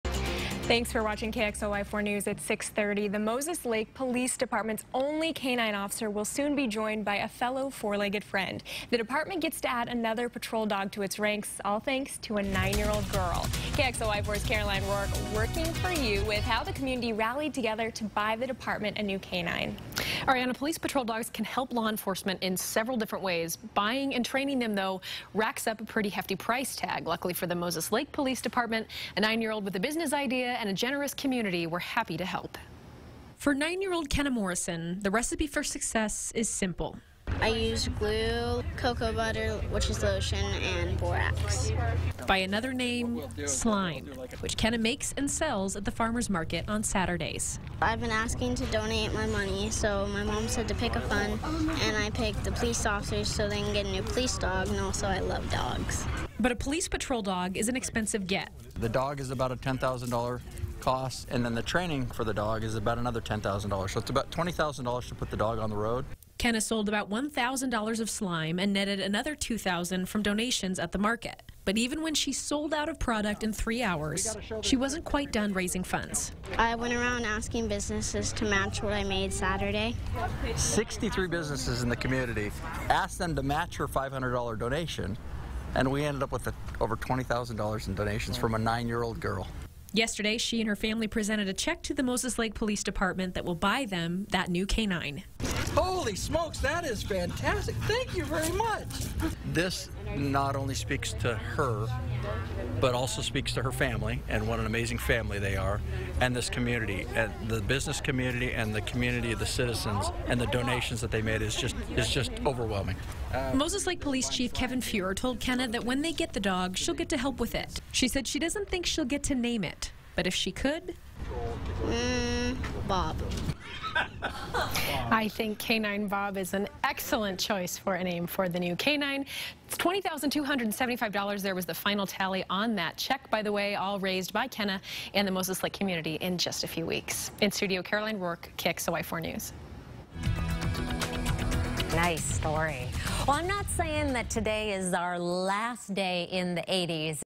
Moses Lake girl's fundraiser nets $20k for new police K9
For 9-year-old Kenna Morrison, the recipe for success is simple. KXLY 4's Caroline Rourke reports.